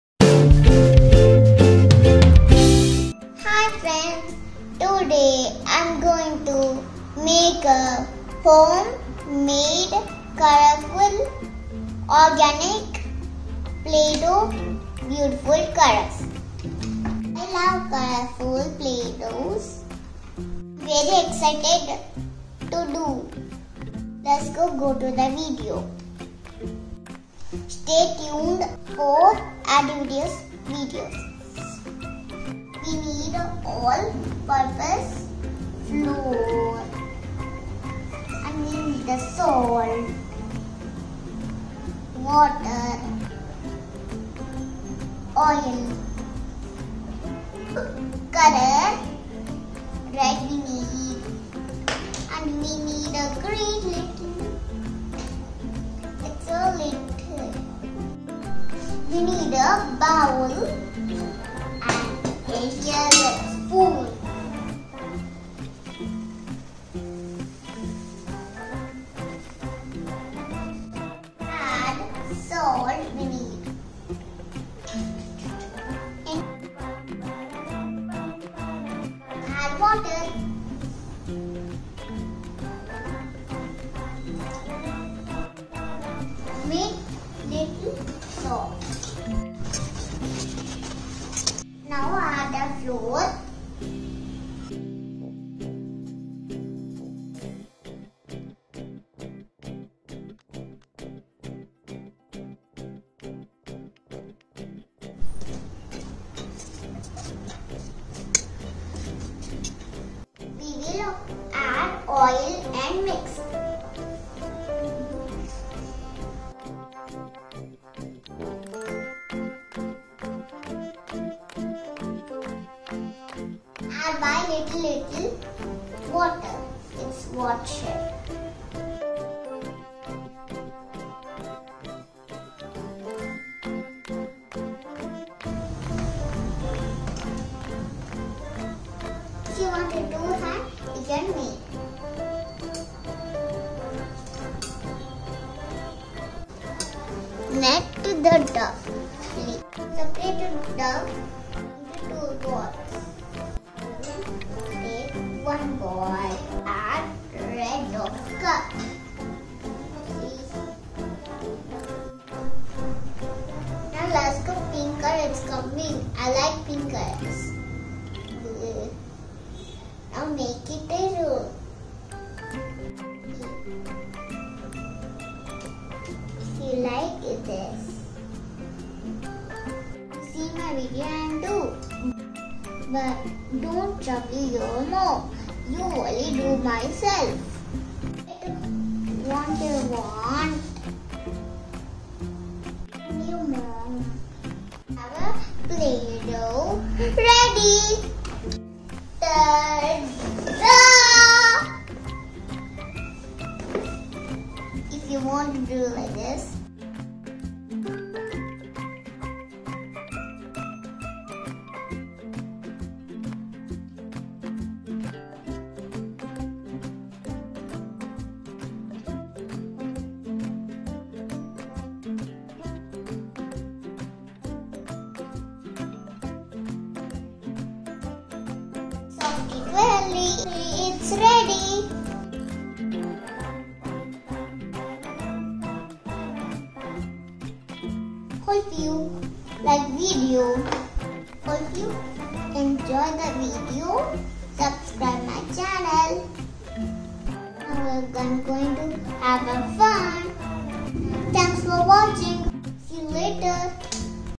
Organic Homemade Play dough
Make your own colourful play dough at home with the simple ingredients. Bonus: Softy ice cream swirl made with the Playdoh kit in this video. Simple preparation and lot of fun. DIY Organic Homemade play dough is ready to play. Enjoy..